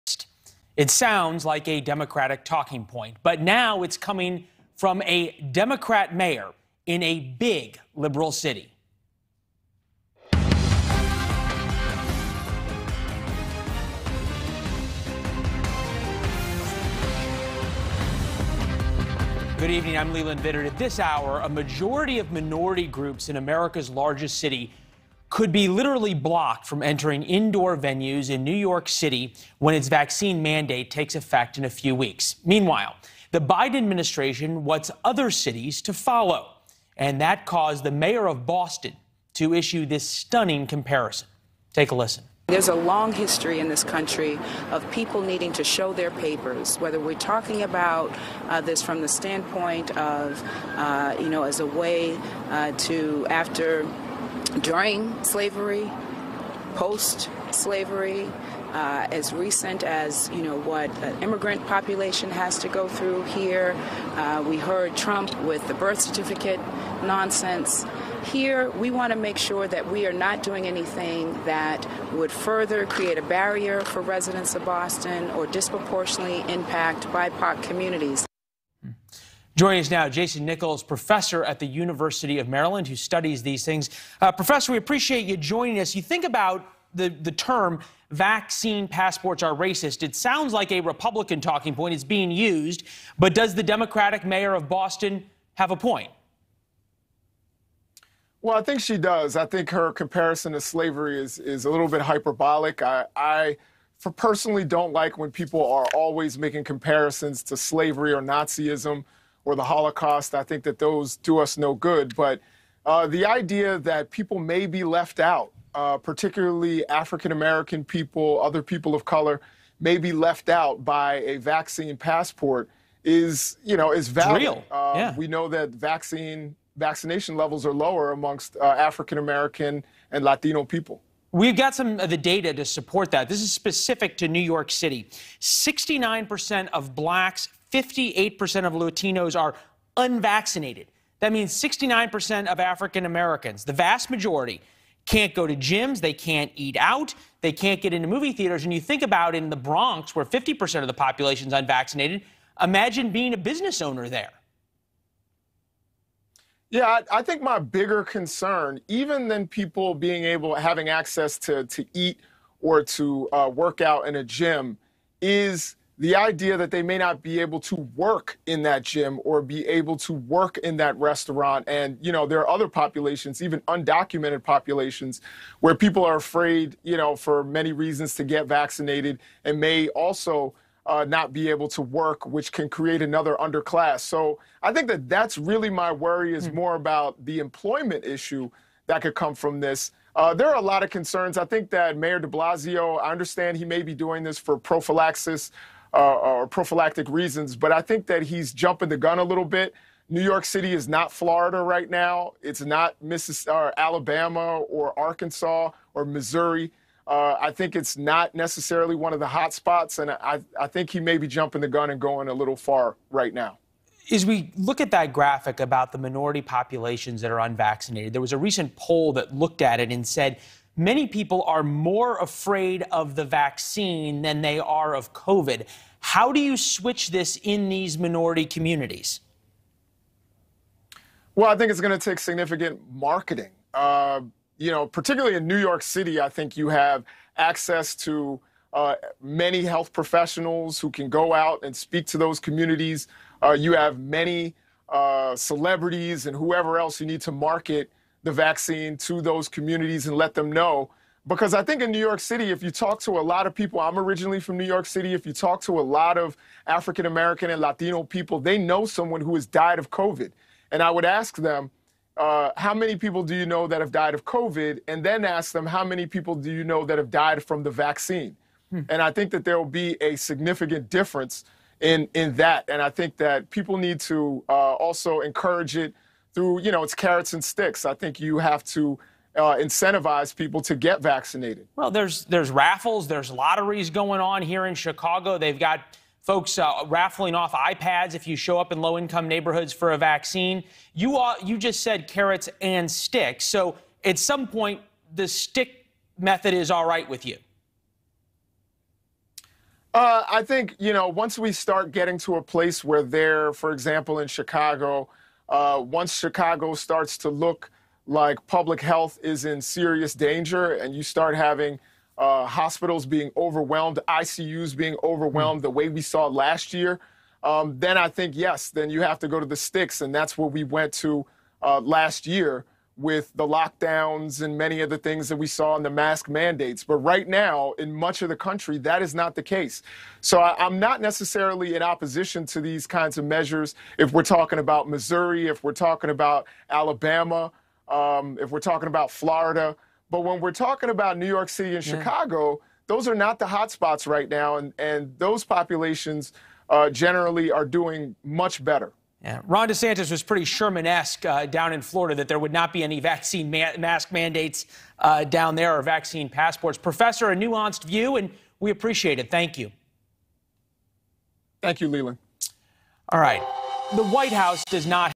New York City Mask Mandates Restricts Minority Group
Leland is joined by Jason Nichols to discuss the ramifications of the New York City mask mandate